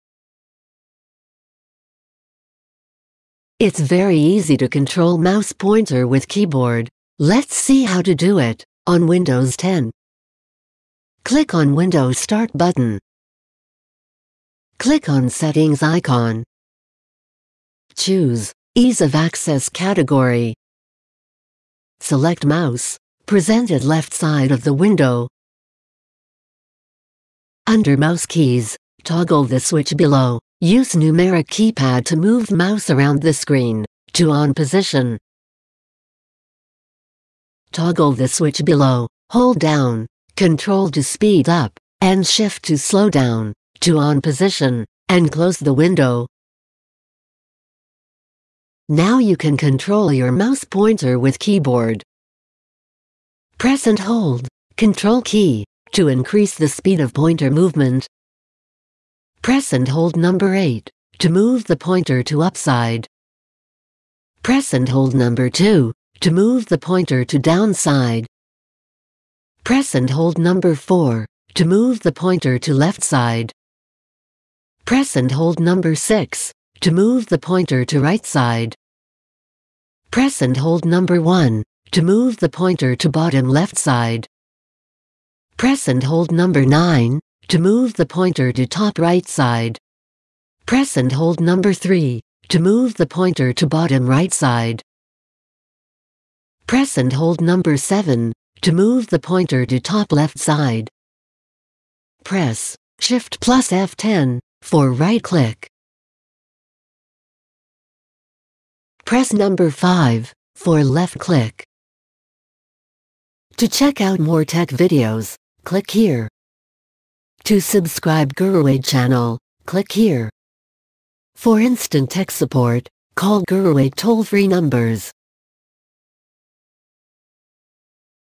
How to Control Mouse Pointer With Keyboard on Windows 10 - GuruAid
GuruAid channel provides you simple & easy steps of How to Control Mouse Pointer With Keyboard on Windows 10.
Windows 10 has the feature called “Mouse Keys” allows you to control mouse with keypad, use the numeric keypad to move your mouse around, click and double-click on items, and even drag and drop. We’ll show you how to turn on and configure Mouse Keys so you can use the numeric keypad as a mouse. You can control the mouse with the help of keyboard. Press and hold, ctrl key, to increase the speed of pointer movement, Press and hold number 8, to move the pointer to up side, 2 for down side, 4 for left side, 6 for right side, 1for bottom left side, 3 for bottom right side, 9 for top-right side, 7for top-left side, + F10, for right click, 5 for left click.  You can do these changes from “Ease of access centre